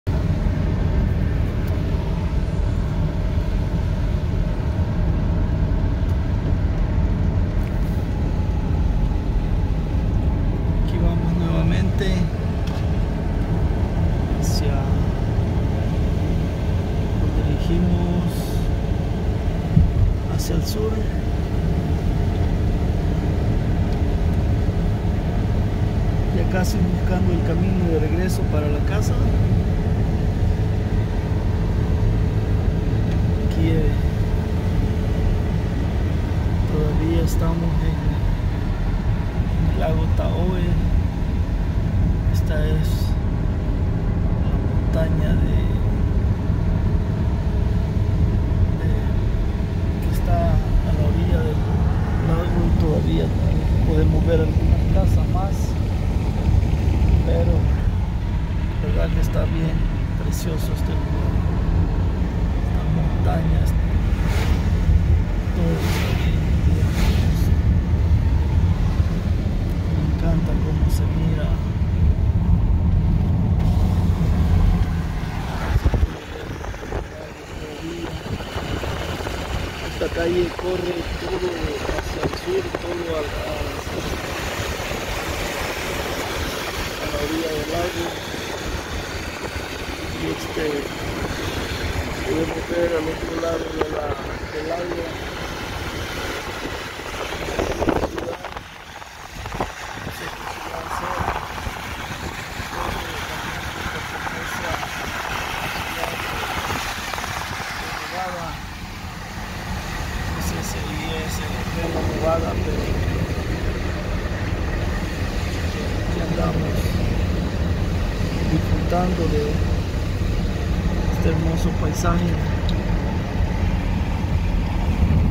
Best Places in Ca, Tahoe Lake
Viajando en la montaña un dia lluvioso